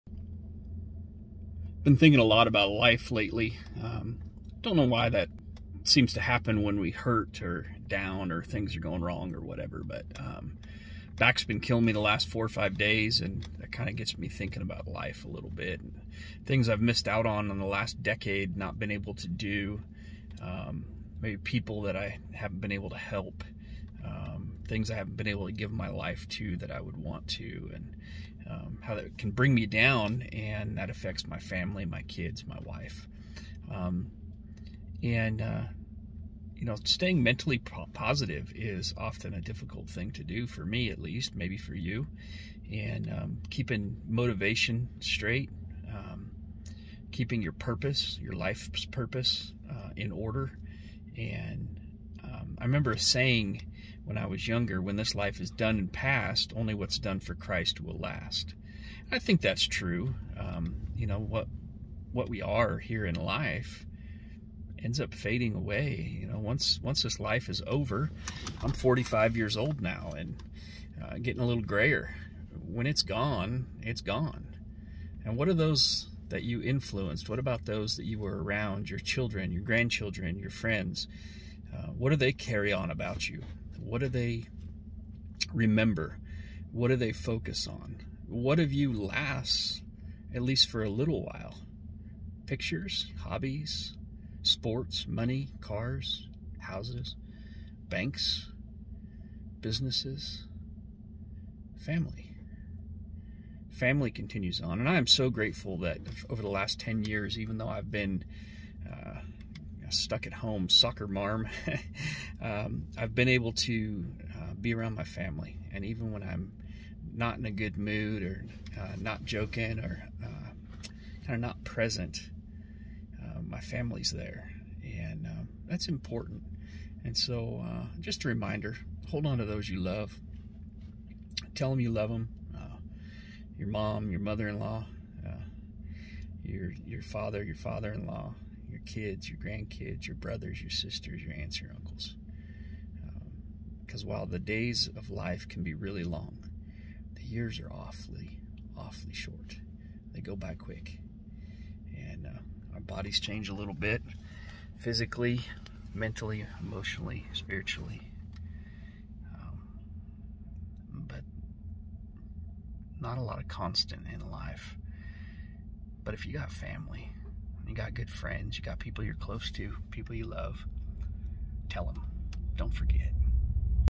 Today's thought
Waiting for Jazz to get off work & just talking about friends, family, legacy, purpose